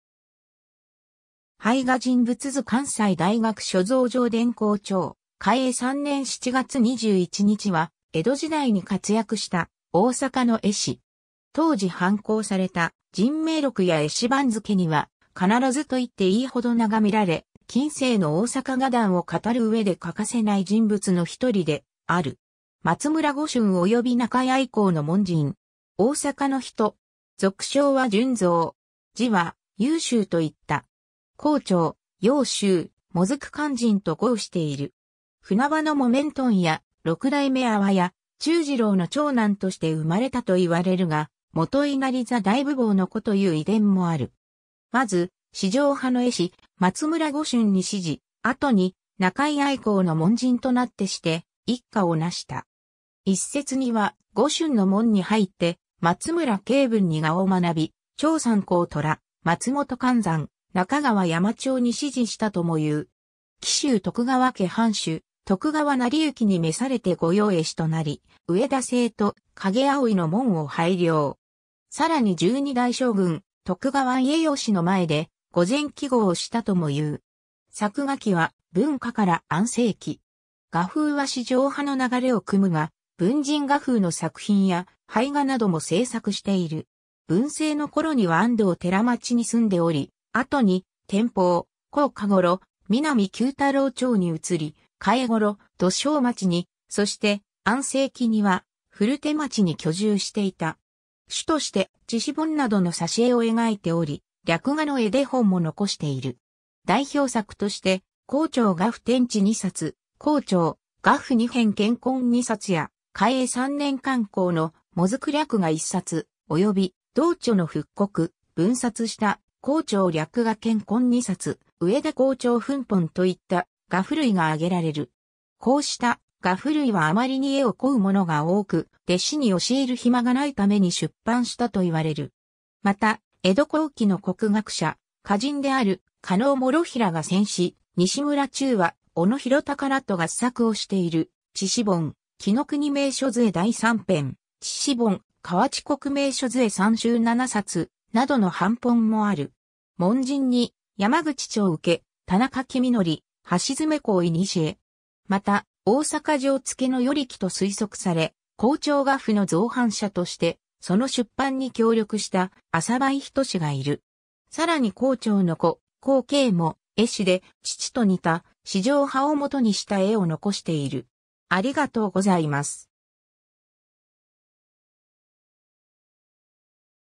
上田公長
「俳画人物図」 関西大学所蔵 上田 公長（うえだ こうちょう、天明8年（1788年） - 嘉永3年7月21日（1850年8月28日））は、江戸時代に活躍した大坂の絵師。
当時板行された人名録や絵師番付には、必ずといっていい程名が見られ、近世の大阪画壇を語る上で欠かせない人物の一人である。
 松村呉春及び中井藍江の門人。
大坂の人。
俗称は順蔵。
字は有秋といった。
公長、雍州、水雲閑人と号している。
船場の木綿問屋・六代目阿波屋忠次郎の長男として生まれたと言われるが、元稲荷座大夫某の子という異伝もある。
まず、四条派の絵師・松村呉春に師事、後に中井藍江の門人となってして一家を成した。
一説には、呉春の門に入って松村景文に画を学び、長山孔寅、松本観山、中川山長に師事したともいう。
紀州徳川家藩主・徳川斉順に召されて御用絵師となり、上田姓と陰葵の紋を拝領。
更に十二代将軍・徳川家慶の前で御前揮毫をしたともいう。
 作画期は、文化（1804年‐1818年）から安政（1854年‐1860年）期。
画風は四条派の流れを汲むが、文人画風の作品や、俳画なども制作している。
文政（1818年‐1830年）の頃には安堂寺町に住んでおり、後に天保（1830年‐1844年）、弘化（1844年‐1848年）頃、南久太郎町に移り、嘉永（1848年‐1854年）頃、道修町に、そして安政期には古手町に居住していた。
主として地誌本などの挿絵を描いており、略画の絵手本も残している。
 代表作として、『公長画譜』天地二冊（天保5年（1834年））、『公長画譜 二編』乾坤二冊（嘉永2年（1849年）や、嘉永3年（1850年）刊行の『水雲略画』一冊、及び同著の復刻・分冊した『公長略画』乾坤ニ冊（文久3年（1863年））、『上田公長粉本』といった画譜類が挙げられる。
こうした画譜類はあまりに絵を乞う者が多く、弟子に教える暇が無いために出版したと言われる。
また、江戸後期の国学者・歌人である加納諸平が撰し、西村中和・小野広隆らと合作をしている地誌本『紀伊国名所図会』第三編（天保9年（1838年））、地誌本『河内国名所図会三集』七冊、などの版本もある。
 門人に山口長受、田中公憲、橋爪公古。
また、大坂城付の与力と推測され、『公長画譜』の蔵版者としてその出版に協力した浅羽韋斎がいる。
更に公長の子、公圭も絵師で、父と似た四条派を元にした絵を残している。